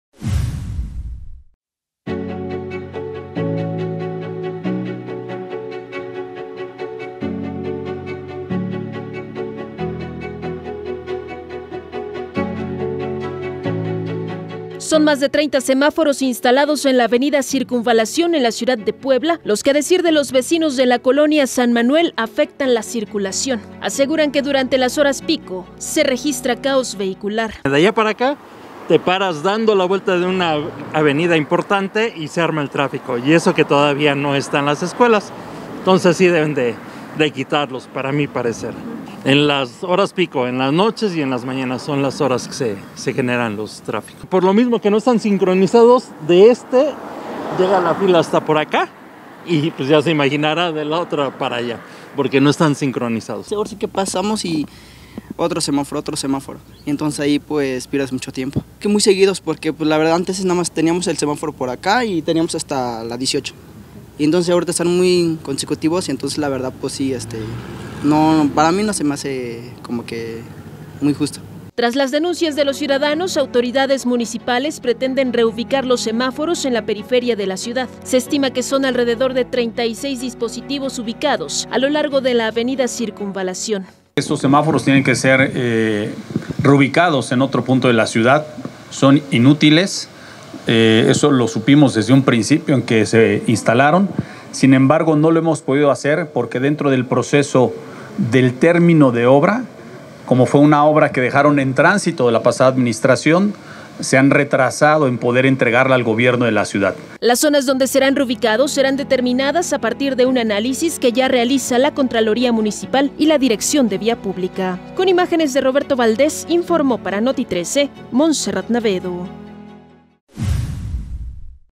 🔴 Reubicarán los 36 semáforos de Avenida CIrcunvalación, en Puebla capital.
🔴 Luego de varias quejas, por fin reubicarán los 36 semáforos de la Avenida Circunvalación, en Puebla capital, los cuales, a decir de lo vecinos y quienes transitan por ahí, solo entorpecen l vialidad.